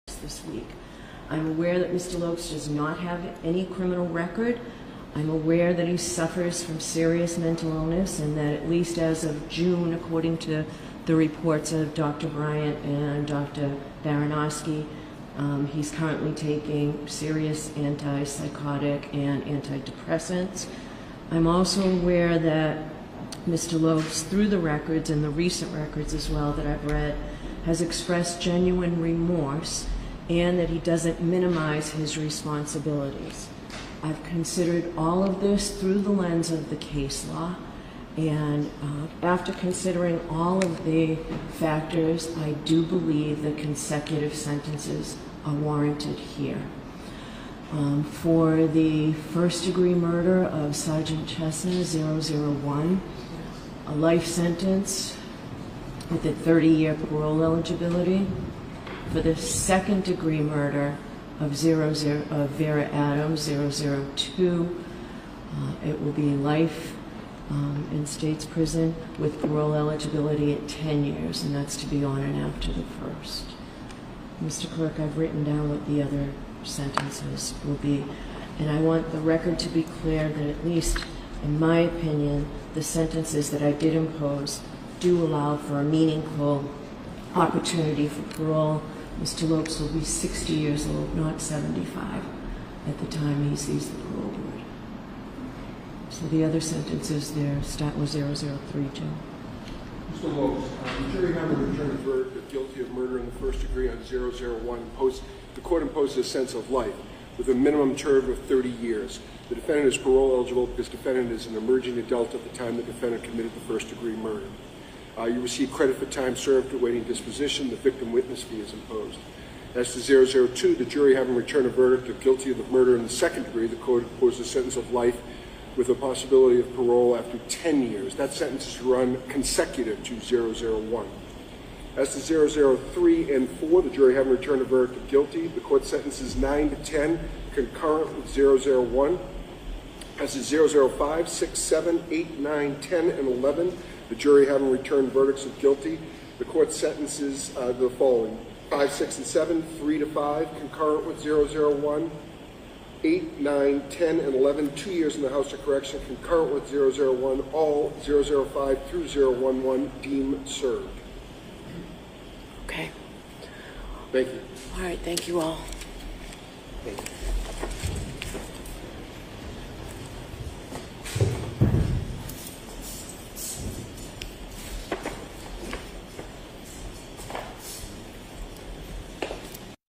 Convicted killer Emanuel Lopes sentenced to life in prison
In February, Emanuel Lopes was found guilty of first-degree murder in the death of Weymouth police Sgt. Michael Chesna and guilty of second-degree murder in the death of Weymouth resident Vera Adams in July 2018.